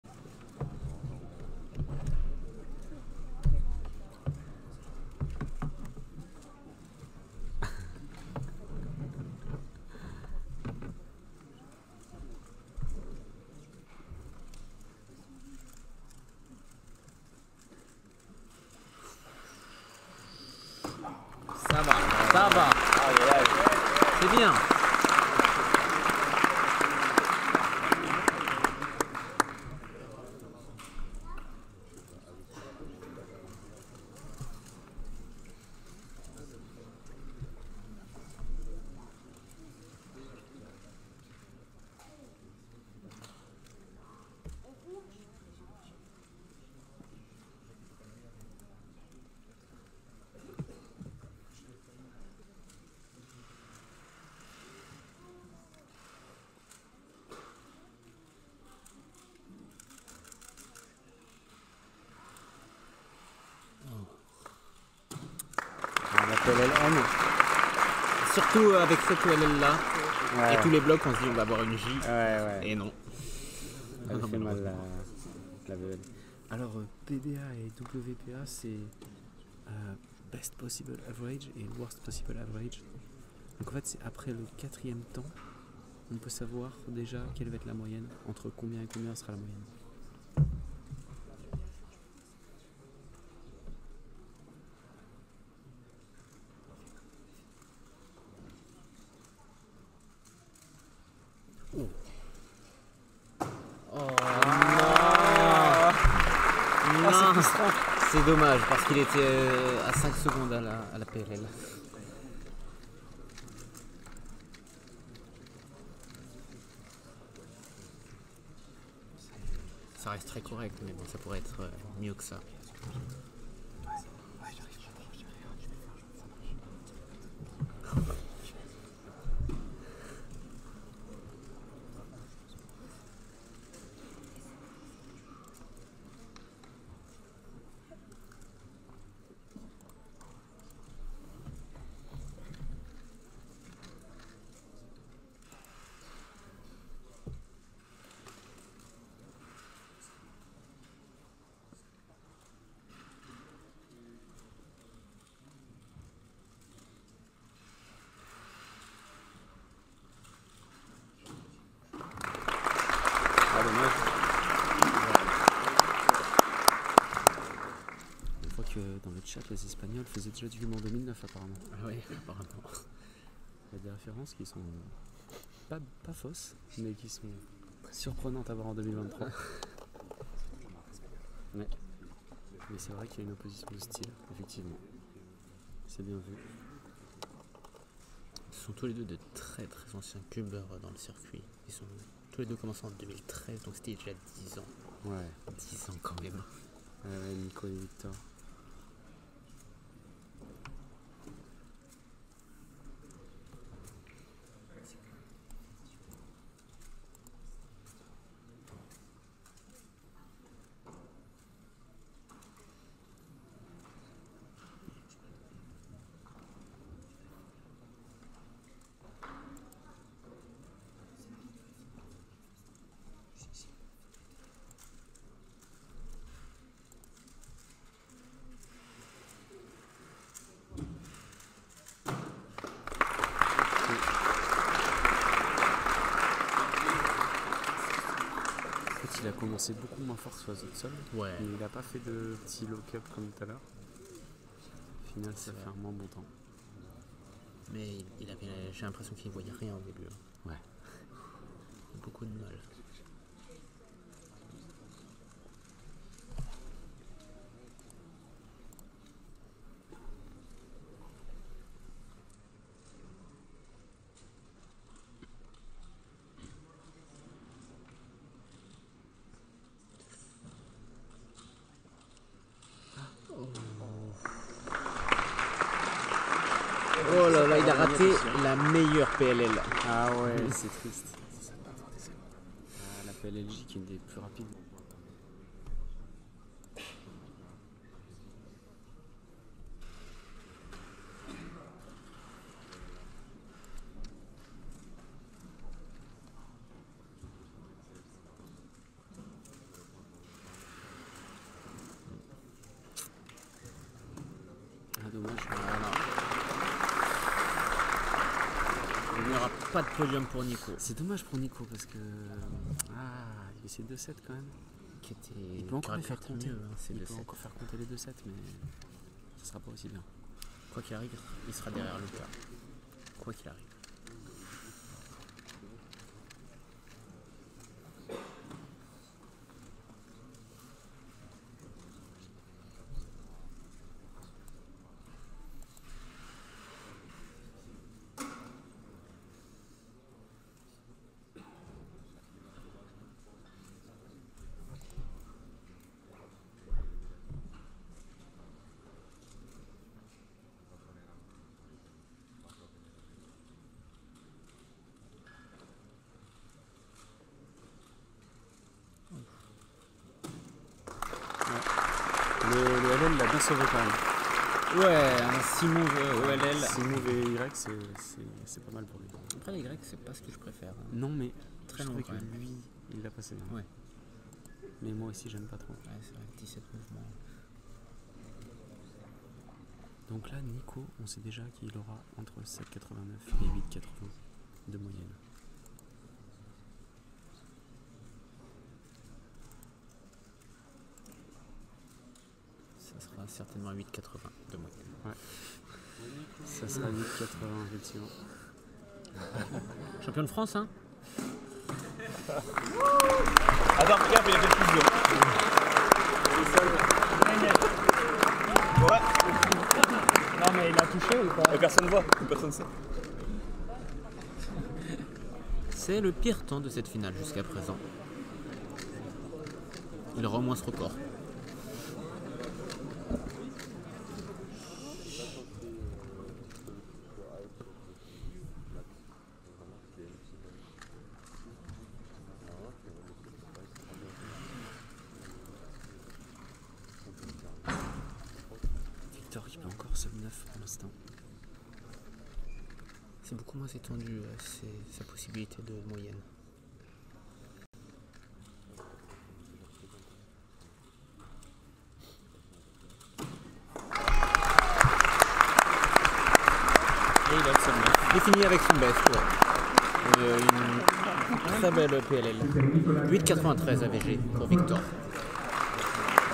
Nicolas Gertner VS Victor Colin ! / Finals French Championship 2023
Salut tout le monde, voilà le 1 contre 1 de la finale des championnats de France. Il s’agit de Victor Colin contre Nicolas Gertner.
Bon visionnage.